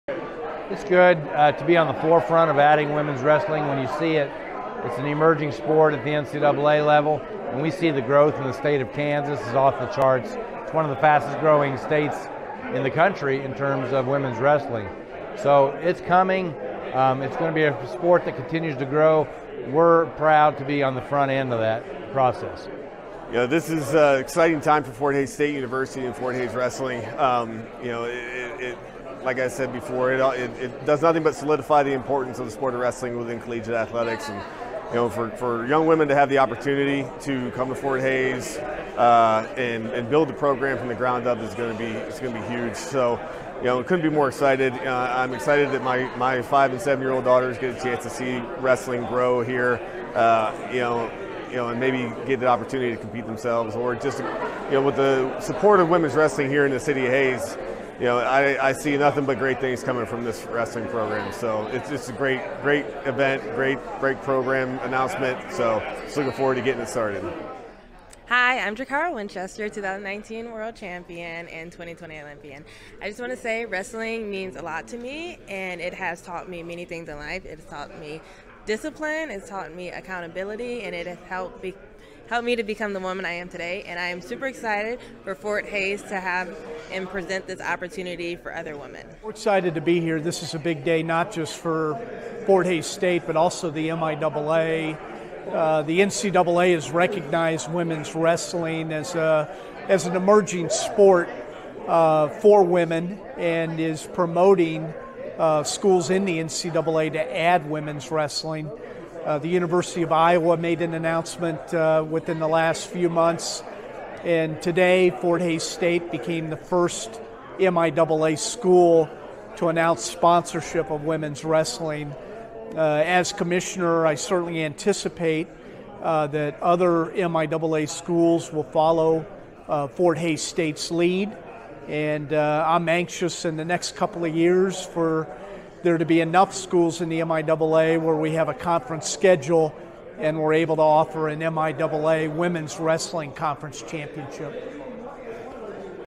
Fort Hays State pins women’s wrestling to its varsity program
Fort Hays State University is adding women’s wrestling to its athletics program, with competition beginning in 2024-25.